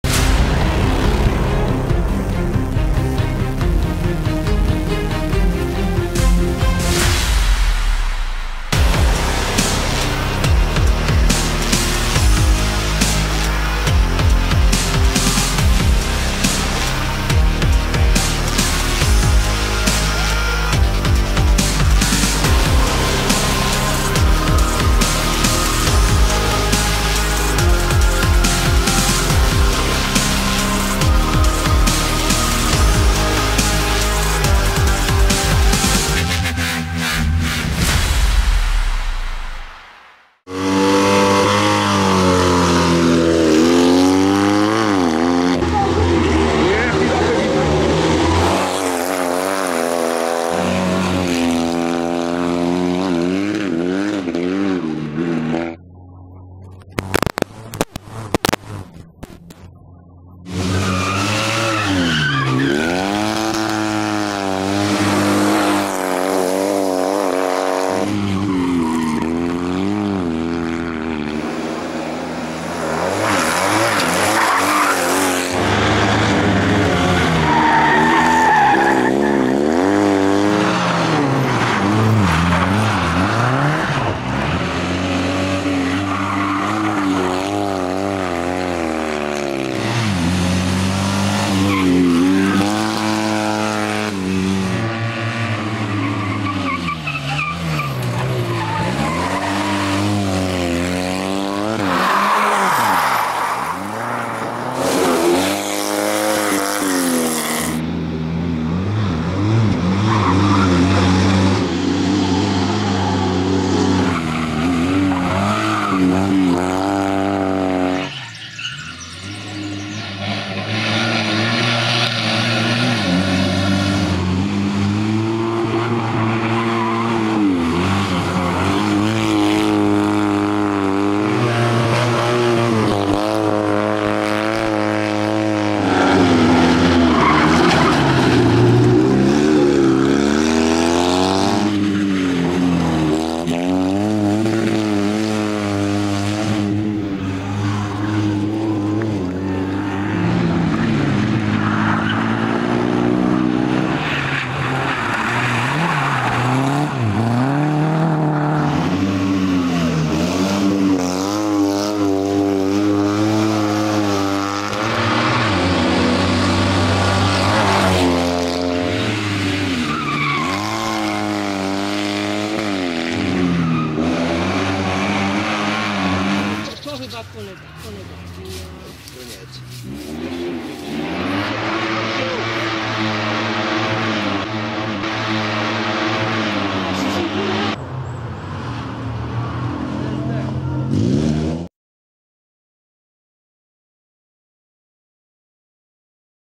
Best of sezon 2016 - Piotr Filapek | FilapekRallyTeam | Fiat 126p
Podsumowanie sezonu 2016 w amatorskich rajdach samochodowych.
Team: Filapek Rally Team
Driver: Piotr Filapek
Co-driver: Bartek Filapek
Car: Fiat 126p / Peugeot 205
Edit: JarekVideoHD / JVHD